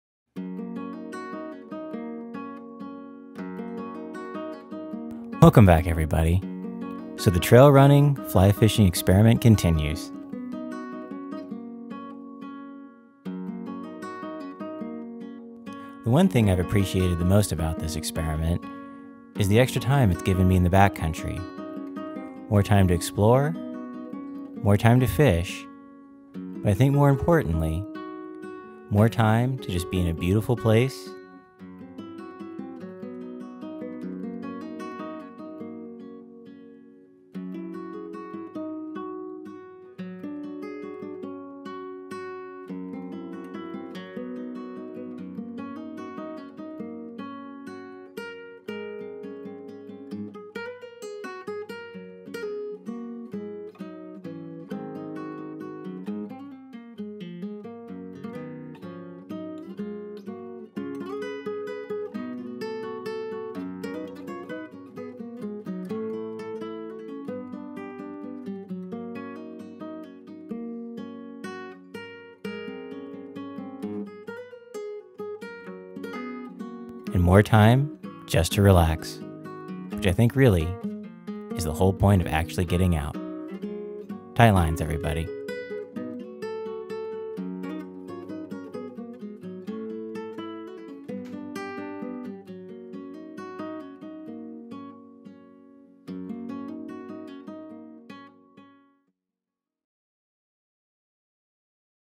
Fly Fishing and Trail Running II
Still seeing where this is going. Hope you like it. The fish at the end are pre-spawn cutts at 12,000 ft moving into the outlet. Only filmed them, didn't fish for them.